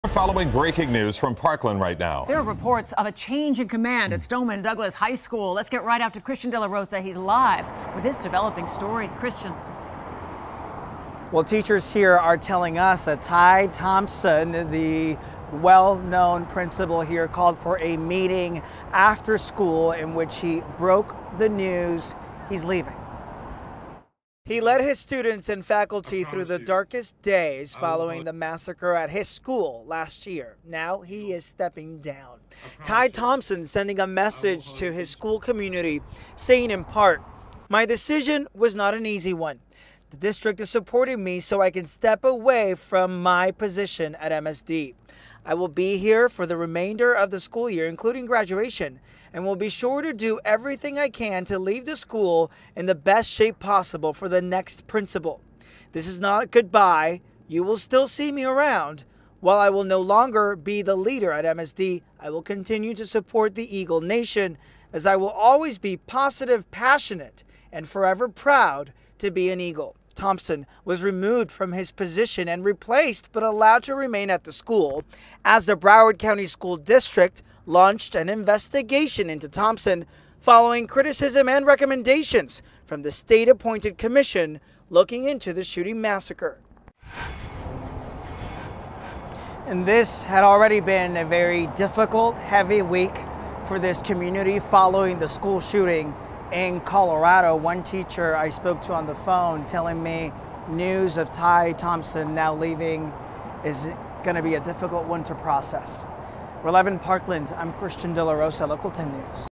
Marjory Stoneman Douglas principal to step down
Ty Thompson has stepped down from his post as the principal of Marjory Stoneman Douglas High School in Parkland.